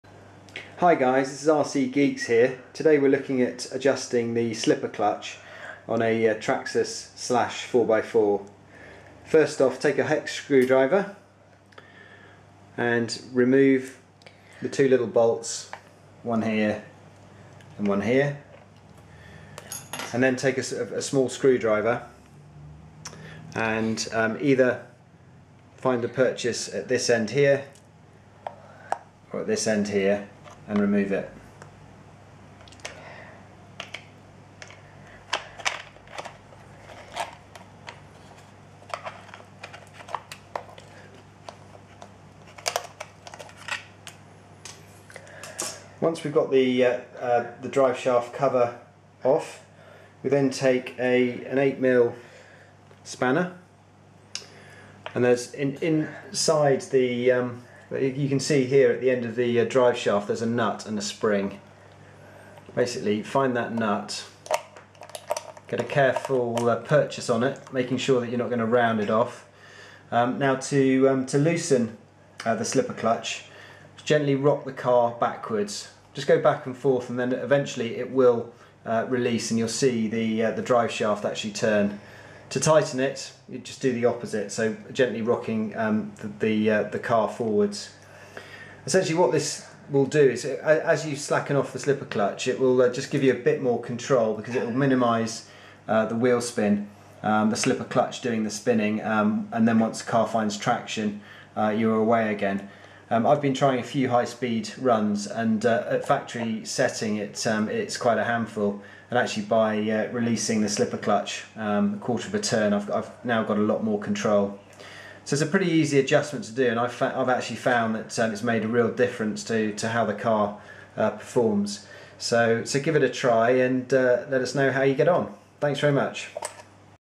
Traxxas Slash 4x4 slipper clutch adjustment
Traxxas Slash 4x4 slipper clutch adjustment. Fine tuning the slipper clutch gives you more control for different driving conditions and surfaces. A free adjustment that makes a big difference!